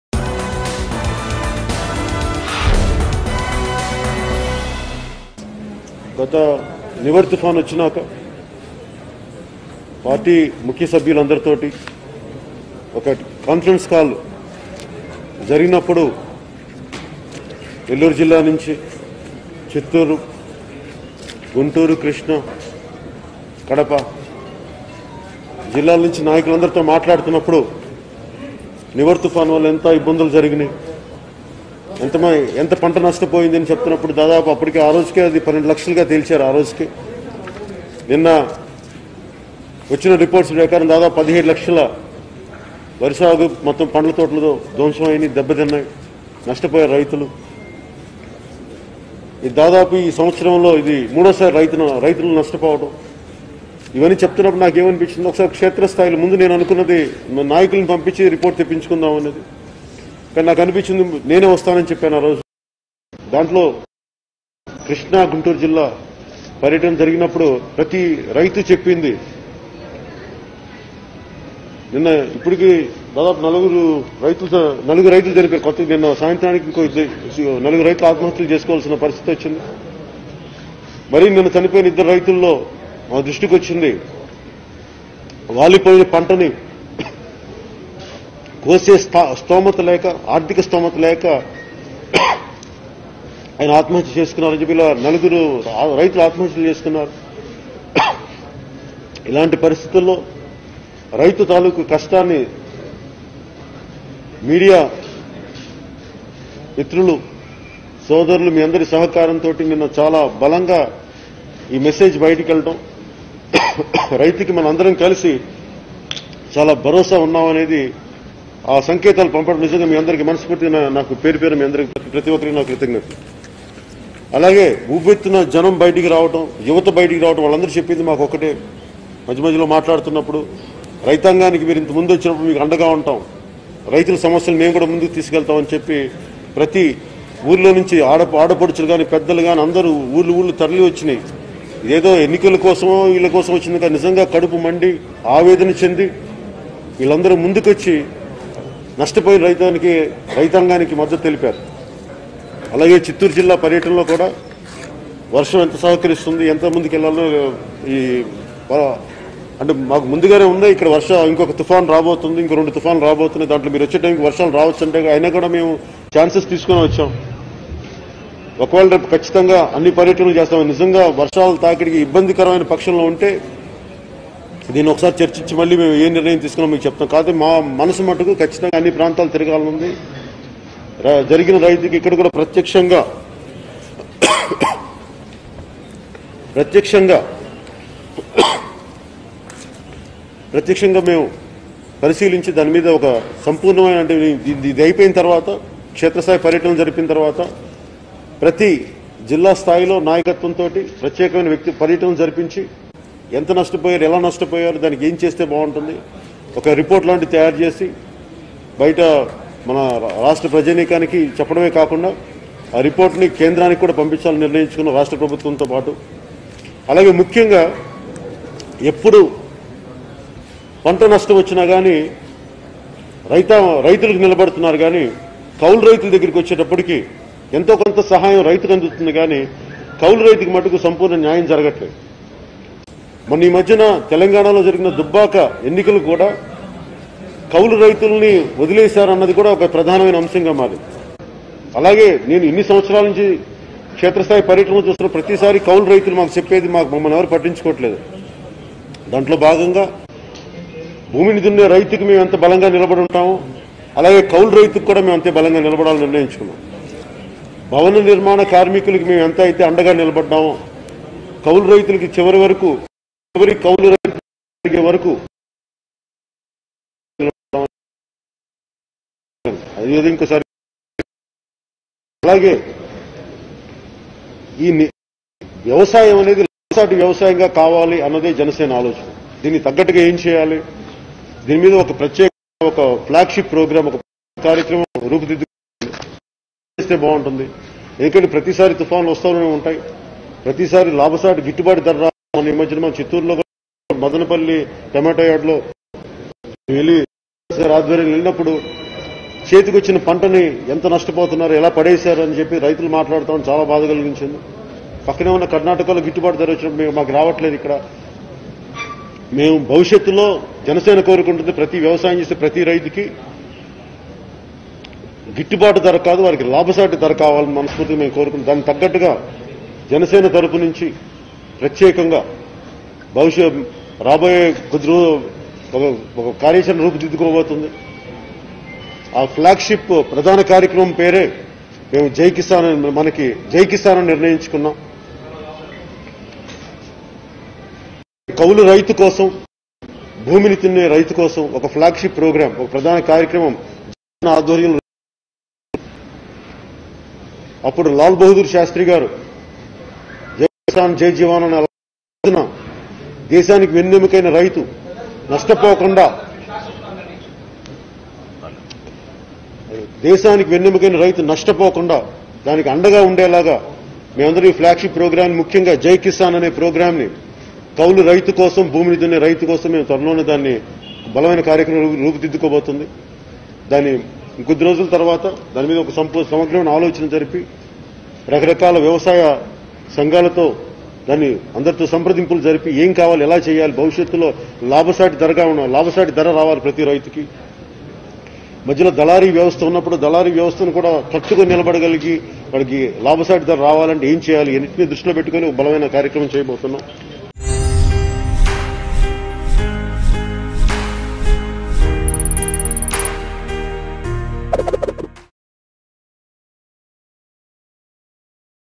Pawan Kalyan Press Meet On Farmers Problems In AP | Janasena | AP Latest Updates | Mango News
Pawan Kalyan Press Meet On Farmers Problems In AP.